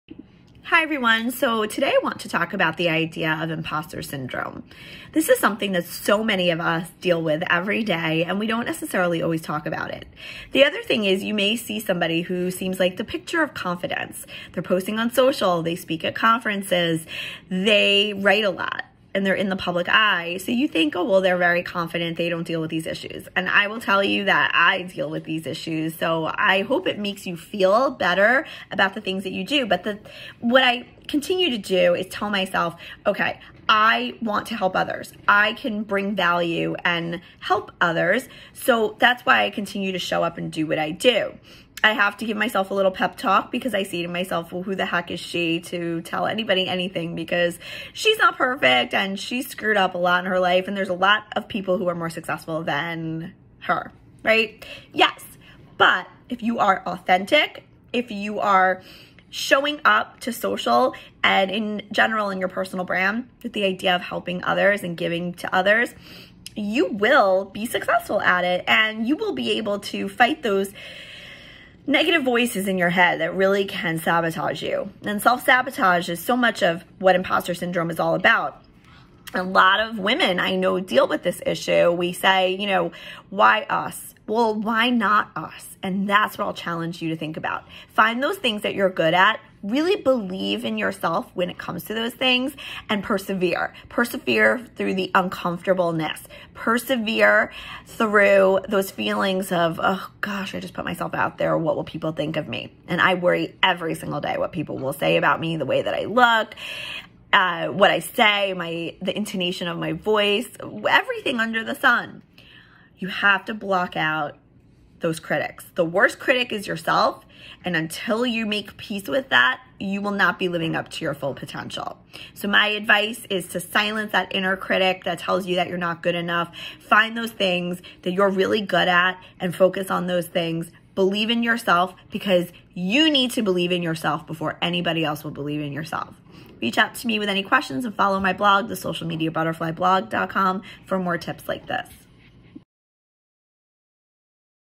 How to overcome imposter syndrome and become the best version of yourself
In this video Stefanie Marrone explores how to overcome imposter syndrome and become the best version of yourself